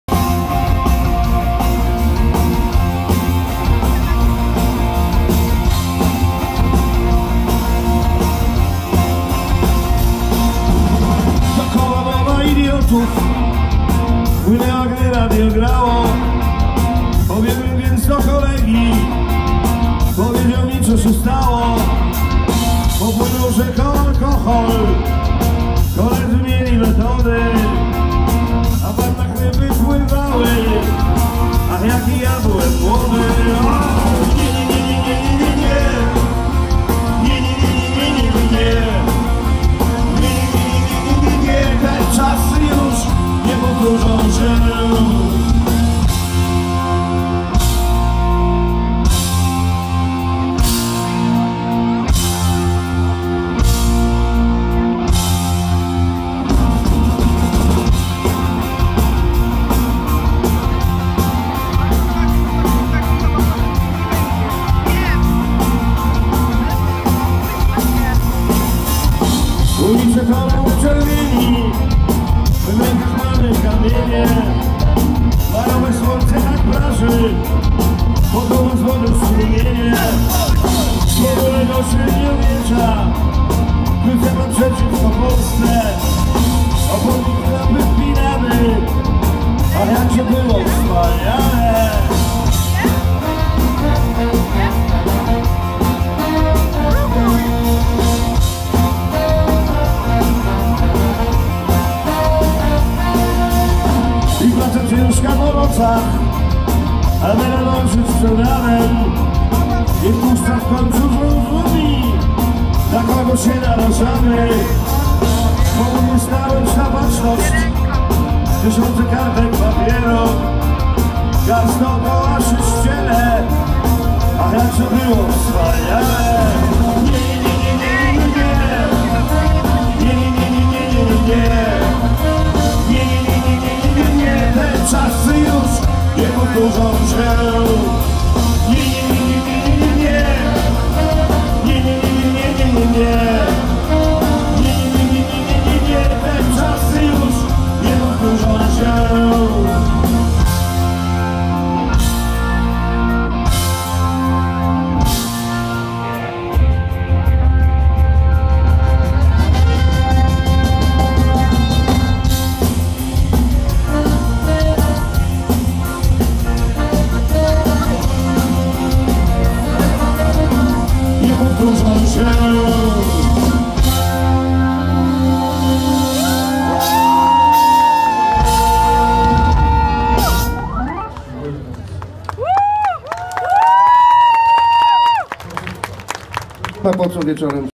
Kult - Próba (Dni Chrzanowa) fragment
Fragment próby zespołu Kult przed koncertem na tegoroczne Dni Chrzanowa